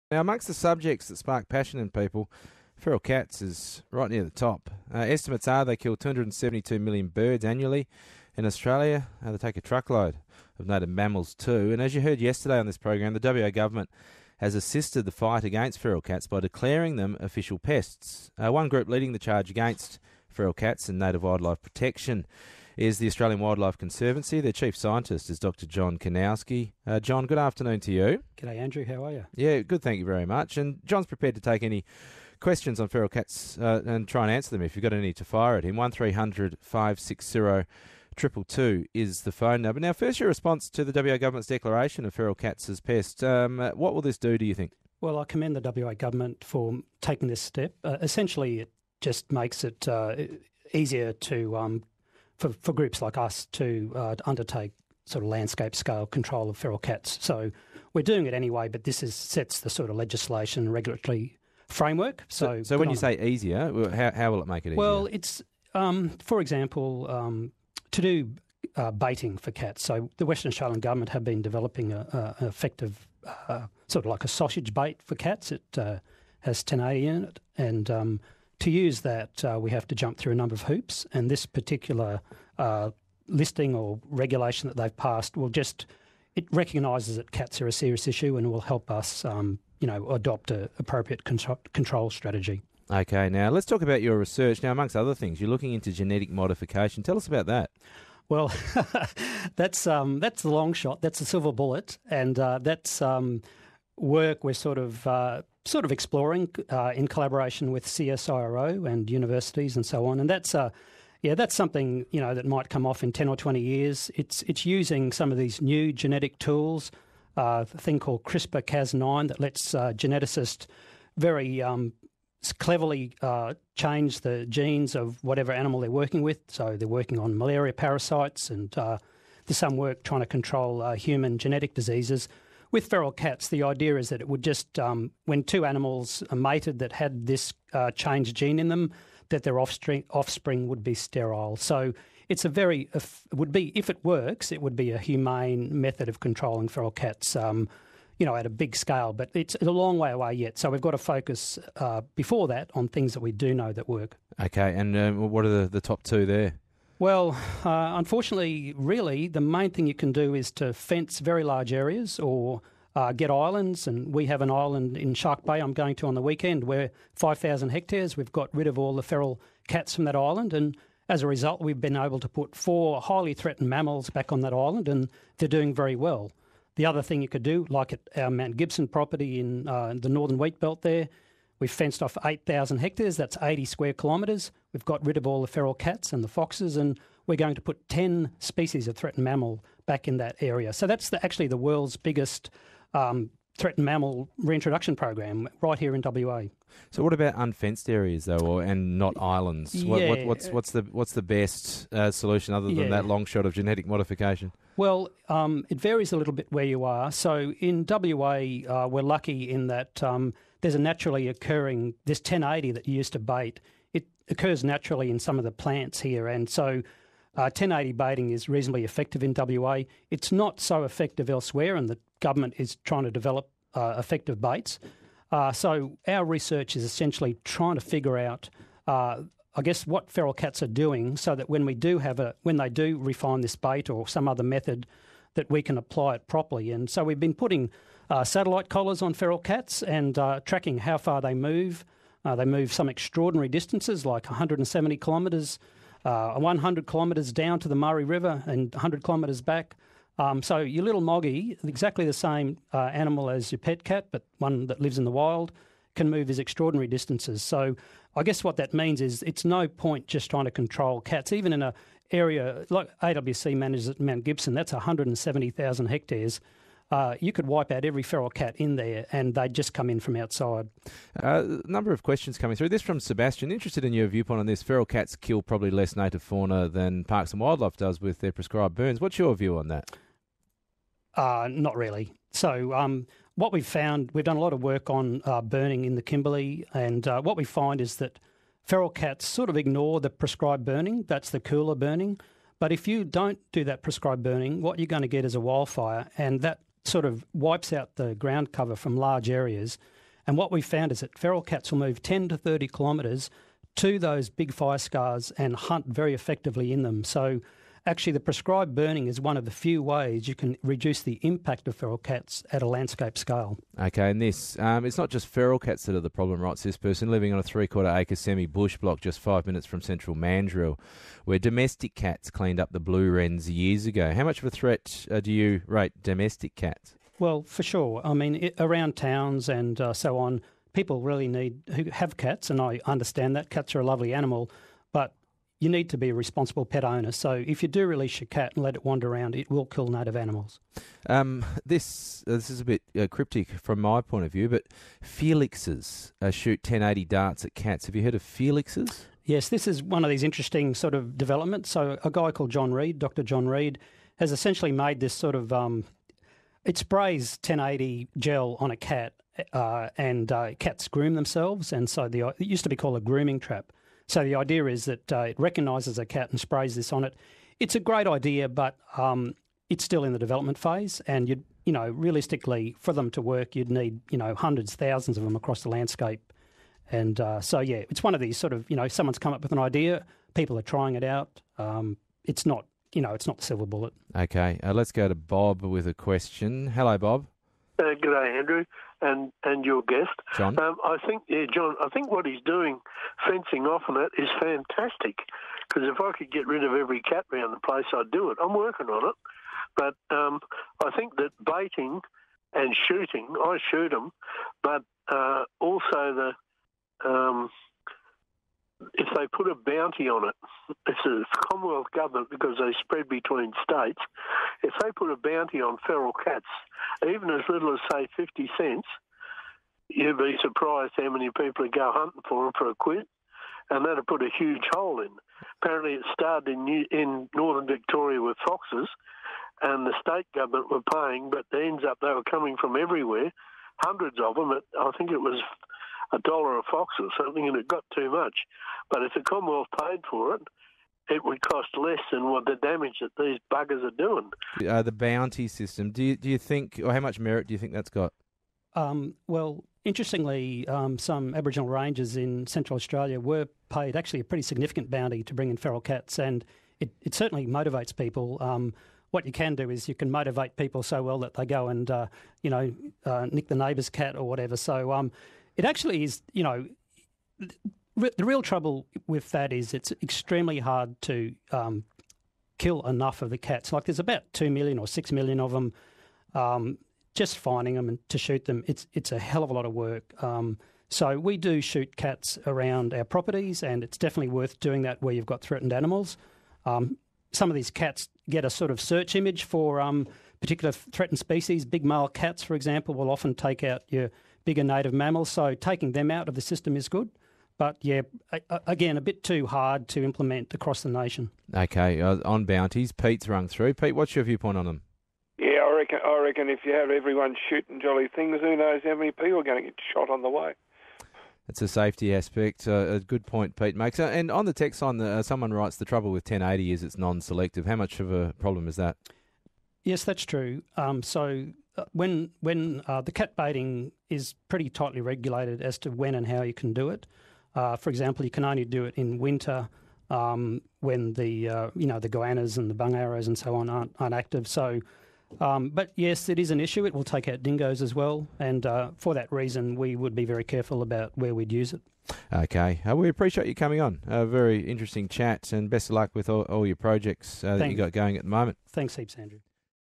Feral cats declared a pest in WA | ABC Radio Interview
Recognised federally as a pest since 2015, feral cats have finally been given the same status in Western Australia.

Our National Science and Conservation Manager, John Kanowski, recently spoke to ABC Radio (WA) regarding the ‘pest’ declaration, discussing how it supports our enormous effort to protect WA’s biodiversity, as well as taking live questions from callers.

Please note that we have supplemented this audio with additional videos and stills from our archives; including footage of feral cats hunting wildlife, which may be distressing for some viewers.

Audio credit: Australian Broadcasting Corporation (ABC)
Video & photos: AWC